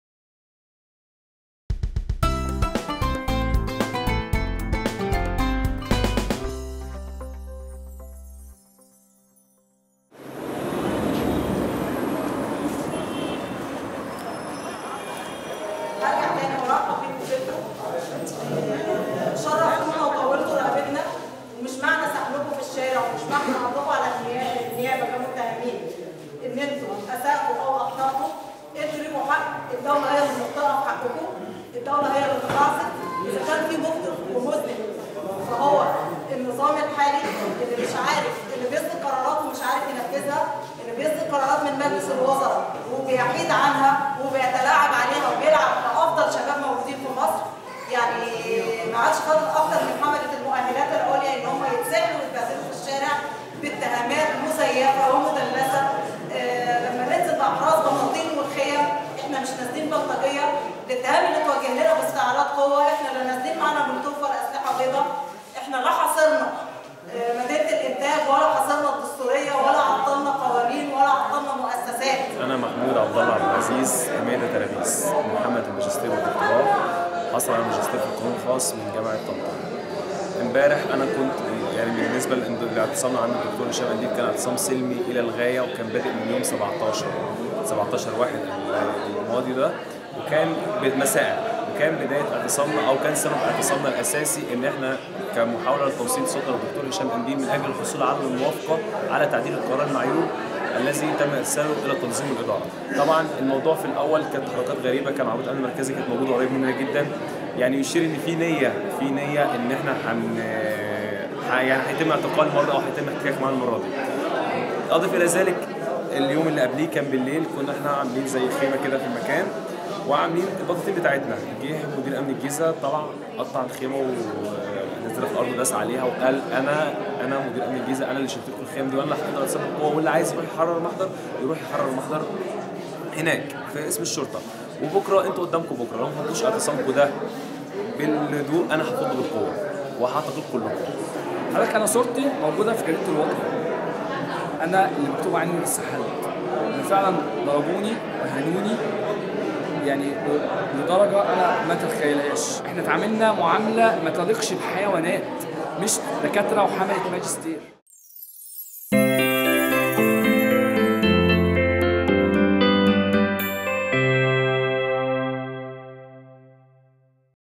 حاملوا الدكتوراه: مدير أمن الجيزة هددنا بالاعتقال
حاملوا الماجستير والدكتوراه : مدير أمن الجيزة هدم خيامنا وهددنا بالاعتقال
جريدة الوطن
جريدة يومية مستقلة